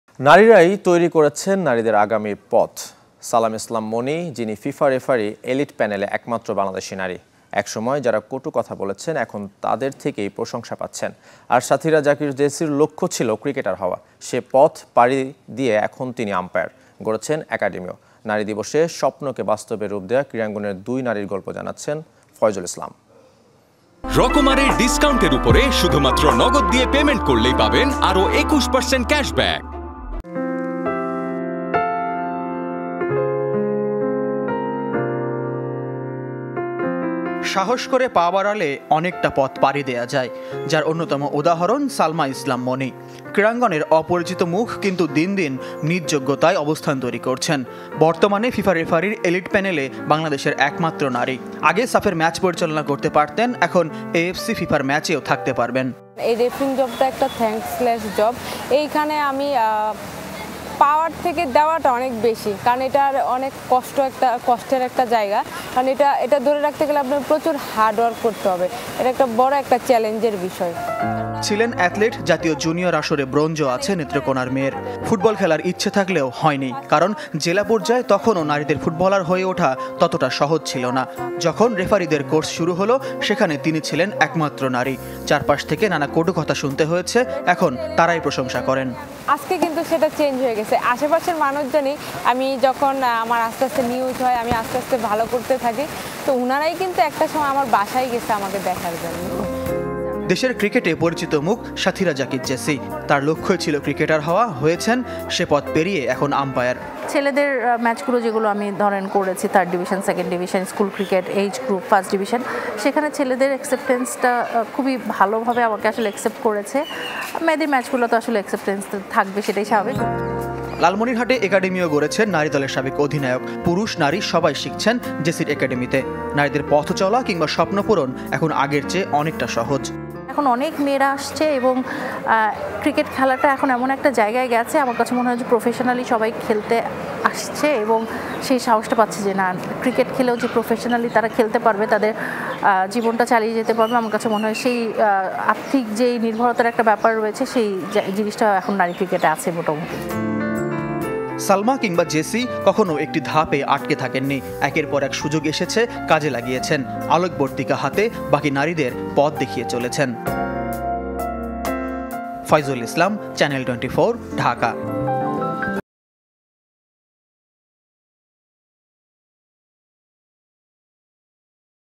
ক্রীড়াঙ্গনে সফল ২ নারীর গল্প | Successful Women Athletes | Moni | Jessy | Sports | Channel 24
নারীরাই তৈরি করছেন নারীদের আগামীর পথ। সালাম ইসলাম মনি, যিনি ফিফা রেফারি এলিট প্যানেলে একমাত্র বাংলাদেশি নারী। এক সময় যারা কটূ কথা বলেছেন, এখন তাদের থেকেই প্রশংসা পাচ্ছেন। আর সাথিরা জাকির জেসির লক্ষ্য ছিল ক্রিকেটার হওয়া। সে পথ পাড়িয়ে এখন তিনি আম্পায়ার। গড়েছেন একাডেমিও। নারী দিবসে, স্বপ্নকে বাস্তবে রূপ দেয়া ক্রীড়াঙ্গনের দুই নারীর গল্প

»» Office Address:
Bangladesh.